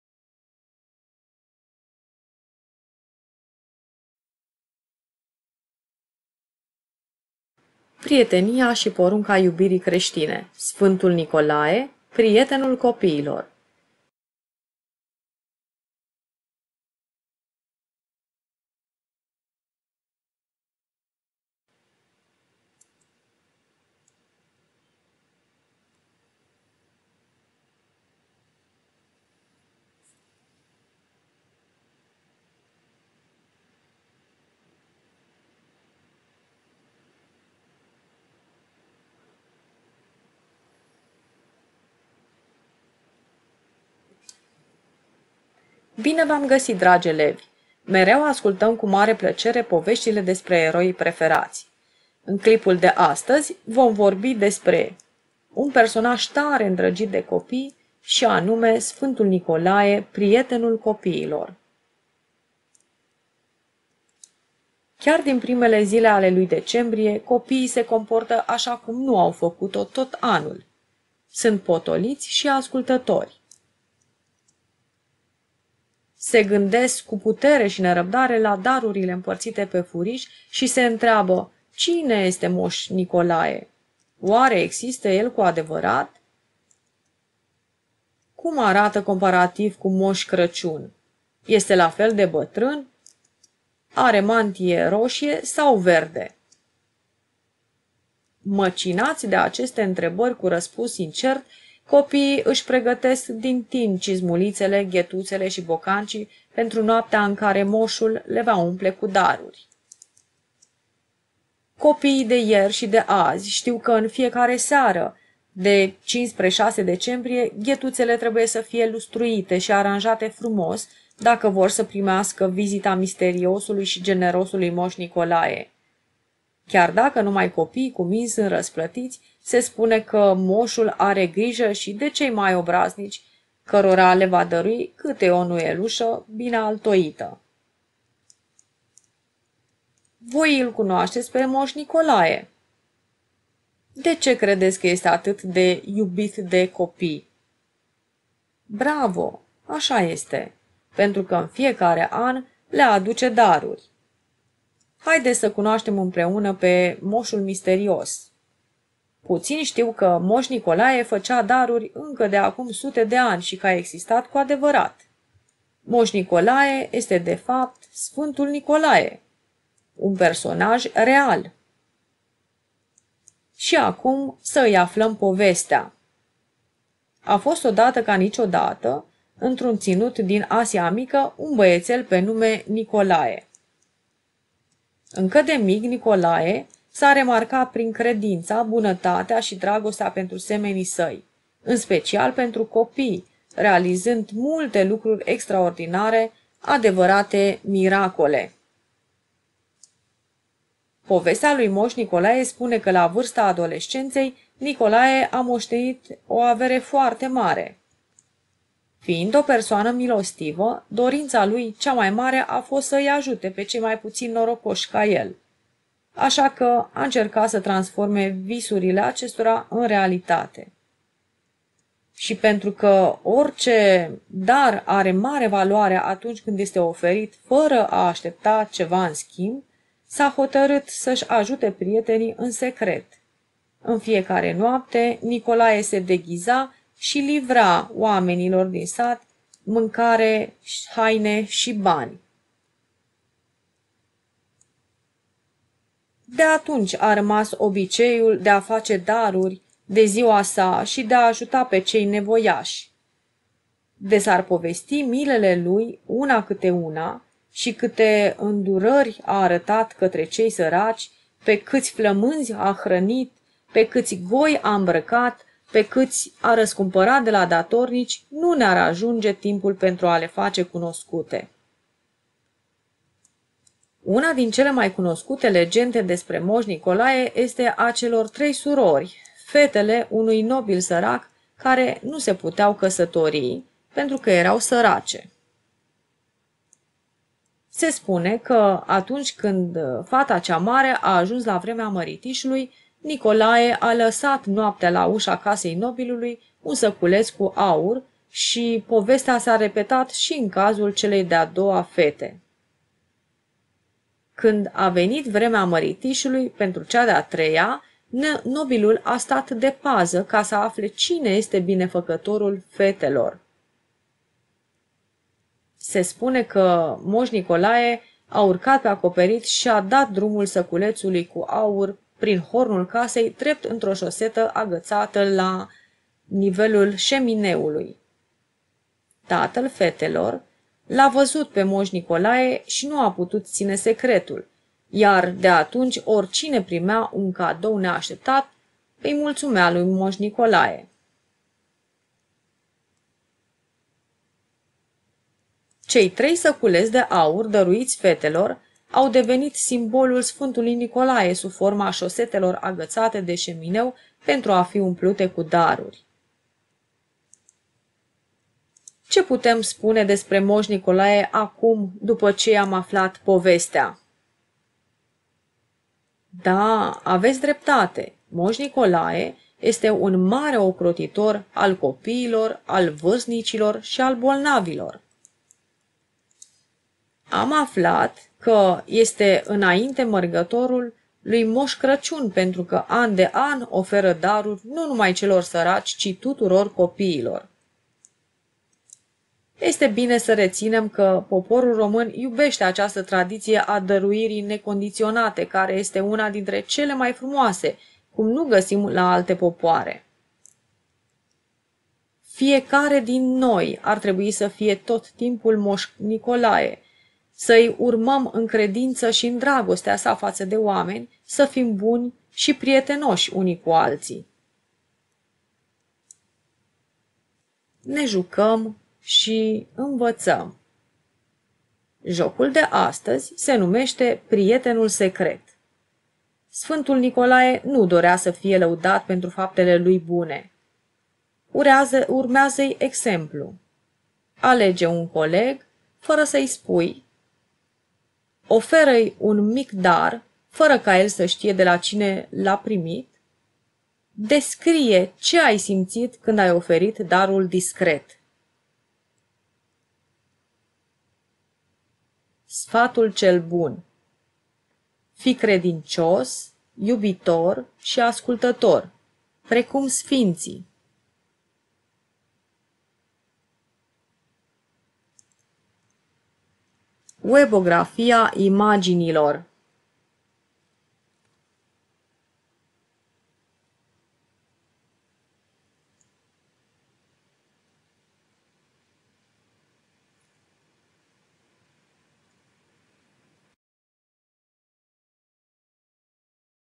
Prietenia și porunca iubirii creștine. Sfântul Nicolae, prietenul copiilor
Resursa conține informații despre persoana Sfântului Nicolae,cunoscut de copii sub numele de Moș Nicolae, Ocrotitorul copiilor, vârstinicilor și bolnavilor.
Resursa începe cu câteva întrebări despre Moș Nicolae și se continuă cu povestea vieții sale, scoțându-se în evidență râvna sa în ajuta dezinteresat aproapele.
În partea imediat următoare este amintită importanța urmării exemplului de credință și bunătate a Sfântului Nicolae și se face îndemnul de a fi și noi Moș Nicolae în orice moment al vieții noastre.
Spre final elevii sunt antrenați să exerseze prin joc cele învățat iar în încheiere este dat un sfat bun.
Scopul urmărit de resursa propusă:
- este de a familiariza elevul cu comportamente ale unui creștin pe baza valorificării exemplului Sfântului Nicolae
- oferă oportunitatea învățării comportamentului diferitelor persoane printr-un joc.

Autor: Carmen Oprea